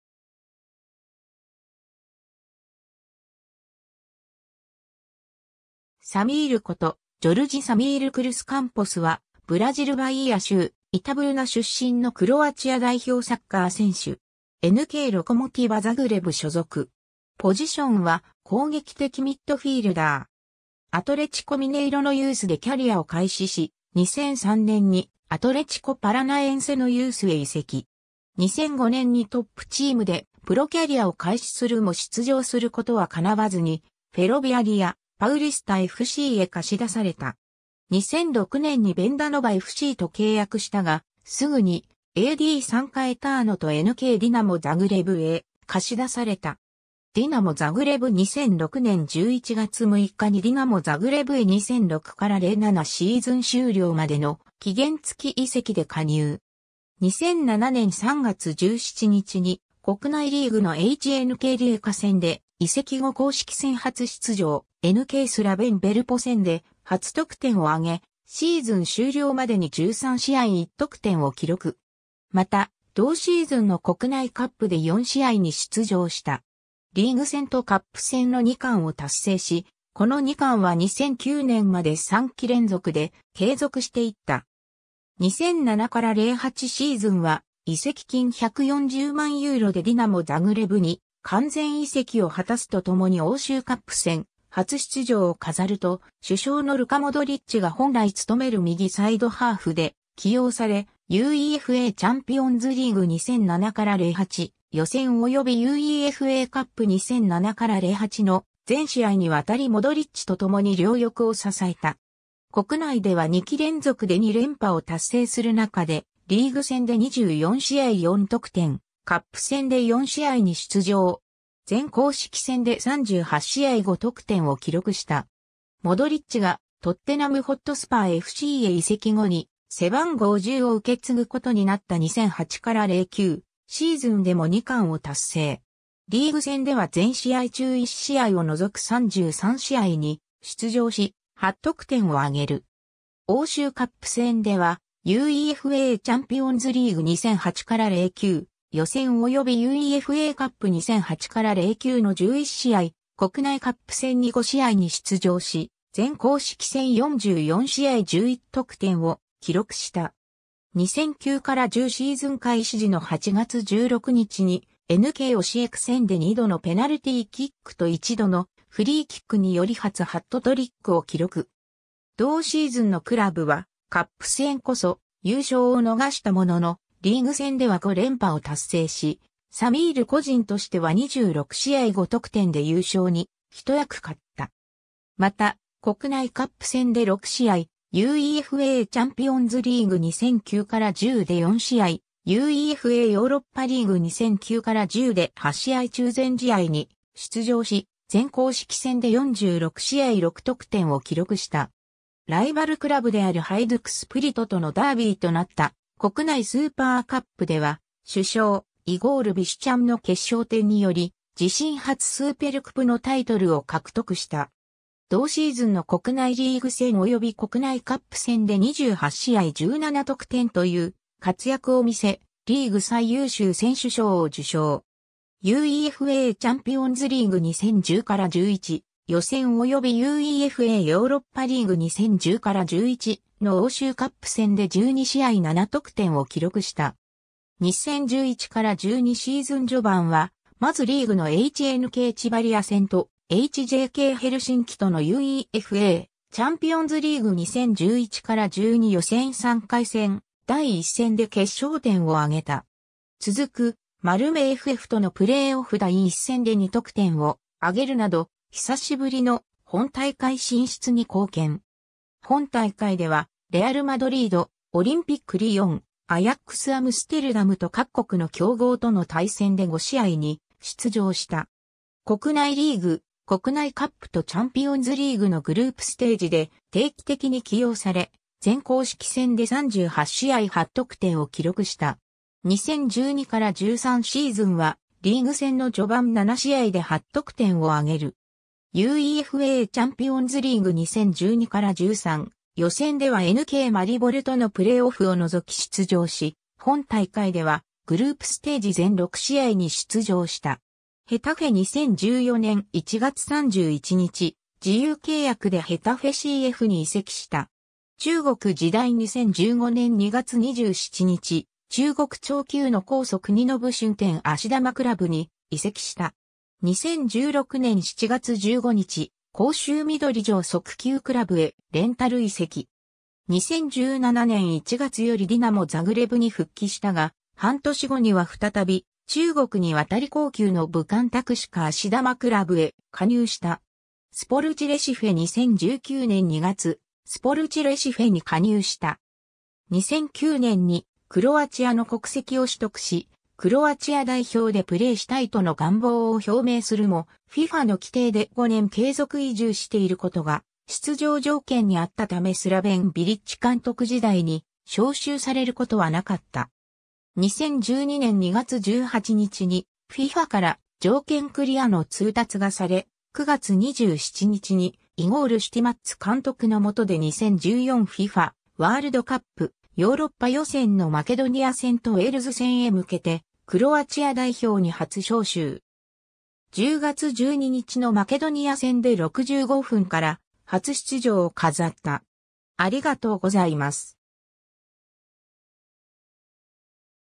ジョルジ・サミール・クルス・カンポス
アトレチコ・パラナエンセの選手
サミール（Sammir）ことジョルジ・サミール・クルス・カンポス（Jorge Sammir Cruz Campos, 1987年4月23日 - ）は、ブラジル・バイーア州イタブーナ出身のクロアチア代表サッカー選手。
NKロコモティヴァ・ザグレブ所属。
ポジションは攻撃的ミッドフィールダー。
 アトレチコ・ミネイロのユースでキャリアを開始し、2003年にアトレチコ・パラナエンセのユースへ移籍。
2005年にトップチームでプロキャリアを開始するも出場することは叶わずに、フェロヴィアリア、パウリスタFCへ貸し出された。
2006年にヴェンダ・ノヴァFCと契約したが、すぐにADサンカエターノとNKディナモ・ザグレブへ貸し出された 。
 ディナモ・ザグレブ 2006年11月6日にディナモ・ザグレブへ2006-07シーズン終了までの期限付き移籍で加入。
2007年3月17日に国内リーグのHNKリエカ戦で移籍後公式戦初出場、NKスラヴェン・ベルポ戦で初得点を挙げ、シーズン終了までに13試合1得点を記録。
また、同シーズンの国内カップで4試合に出場した。
リーグ戦とカップ戦の2冠を達成し、この2冠は2009年まで3季連続で継続していった。
 2007-08シーズンは、移籍金140万ユーロでディナモ・ザグレブに完全移籍を果たすと共に欧州カップ戦初出場を飾ると、主将のルカ・モドリッチが本来務める右サイドハーフで起用され、UEFAチャンピオンズリーグ 2007-08予選及びUEFAカップ 2007-08の全試合に渡りモドリッチと共に両翼を支えた。
国内では2季連続で2連覇を達成する中でリーグ戦で24試合4得点、カップ戦で4試合に出場。
全公式戦で38試合5得点を記録した。
 モドリッチがトッテナム・ホットスパーFCへ移籍後に背番号10を受け継ぐことになった2008-09シーズンでも2冠を達成。
リーグ戦では全試合中1試合を除く33試合に出場し、8得点を挙げる。
欧州カップ戦ではUEFAチャンピオンズリーグ 2008-09予選及びUEFAカップ 2008-09の11試合、国内カップ戦に5試合に出場し、全公式戦44試合11得点を記録した。
 2009-10シーズン開始時の8月16日にNKオシエク戦（ホーム5-0勝利）で2度のペナルティーキックと1度のフリーキックにより初ハットトリックを記録。
同シーズンのクラブは、カップ戦こそ優勝を逃したものの、リーグ戦では5連覇を達成し、サミール個人としては26試合5得点で優勝に一役買った。
また、国内カップ戦で6試合、UEFAチャンピオンズリーグ 2009-10で4試合、UEFAヨーロッパリーグ 2009-10で8試合中全試合に出場し、全公式戦で46試合6得点を記録した。
 ライバルクラブであるハイドゥク・スプリトとのダービー（1-0勝利）となった国内スーパーカップでは、主将イゴール・ビシュチャンの決勝点により、自身初スーペルクプのタイトルを獲得した。
同シーズンの国内リーグ戦及び国内カップ戦で28試合17得点という活躍を見せ、リーグ最優秀選手賞を受賞。
UEFAチャンピオンズリーグ 2010-11予選及びUEFAヨーロッパリーグ 2010-11の欧州カップ戦で12試合7得点を記録した。
 2011-12シーズン序盤は、まずリーグのHNKチバリア戦と、HJKヘルシンキとのUEFAチャンピオンズリーグ 2011-12予選3回戦第1戦（2-1勝利）で決勝点を挙げた。
続くマルメFFとのプレイオフ第1戦（4-1勝利）で2得点を挙げるなど久しぶりの本大会進出に貢献。
本大会ではレアル・マドリード、オリンピック・リヨン、アヤックス・アムステルダムと各国の強豪との対戦で5試合に出場した。
国内リーグ・国内カップとチャンピオンズリーグのグループステージで定期的に起用され、全公式戦で38試合8得点を記録した。
 2012-13シーズンは、リーグ戦の序盤7試合で8得点 (PK6) を挙げる。
UEFAチャンピオンズリーグ 2012-13予選ではNKマリボルとのプレイオフを除き出場し、本大会ではグループステージ全6試合に出場した。
 ヘタフェ 2014年1月31日、自由契約でヘタフェC...